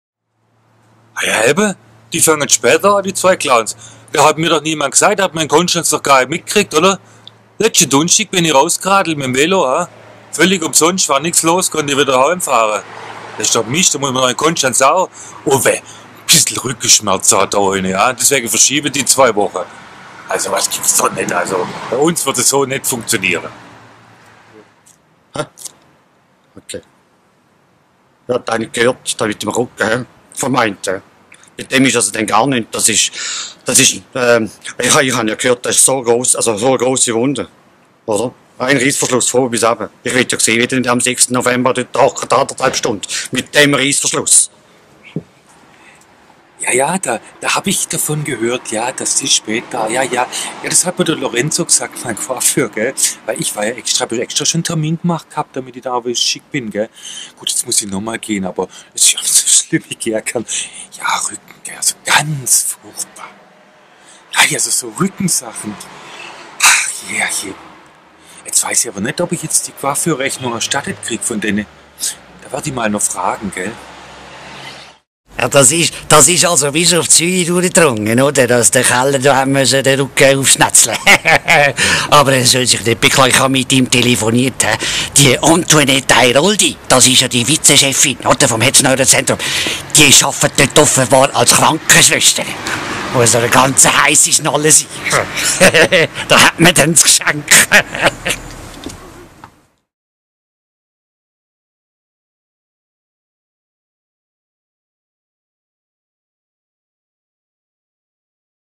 KellerSchuran `14 Trailer
"KellerSchuran - Die Wochenschau"

Start der 5. Staffel  ab 6. November 2014 • 20 Uhr

Restaurant Dreiegg Frauenfeld

Uwe Schuran und Markus Keller